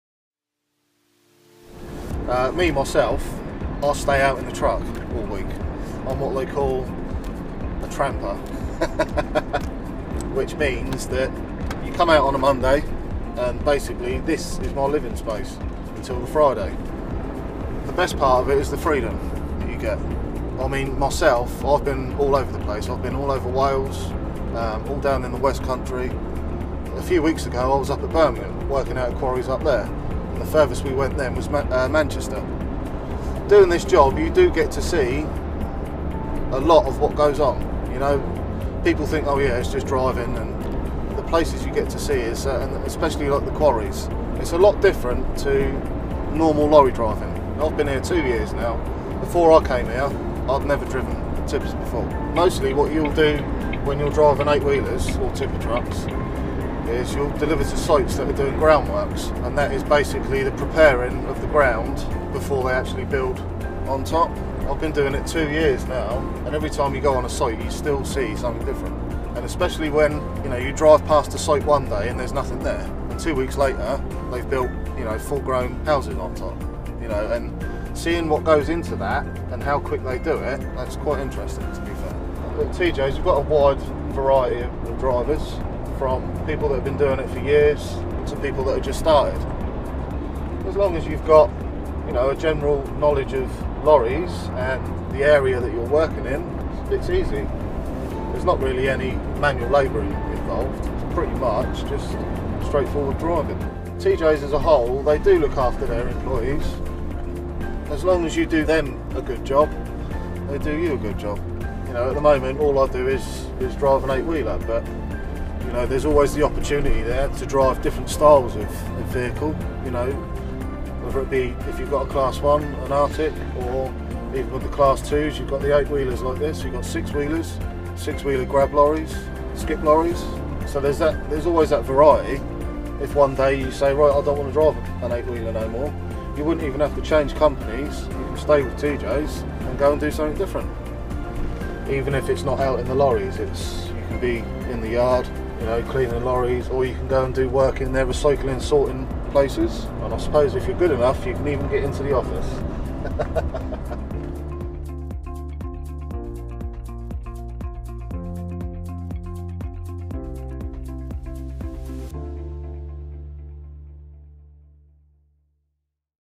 A Day in the Life of a Tipper Truck Driver - TJ Transport
The cameras follow our 8 wheel tipper driver, Chris, as he goes about his day collecting and delivering aggregate, telling us why he enjoys his job, and what he likes about TJ Transport.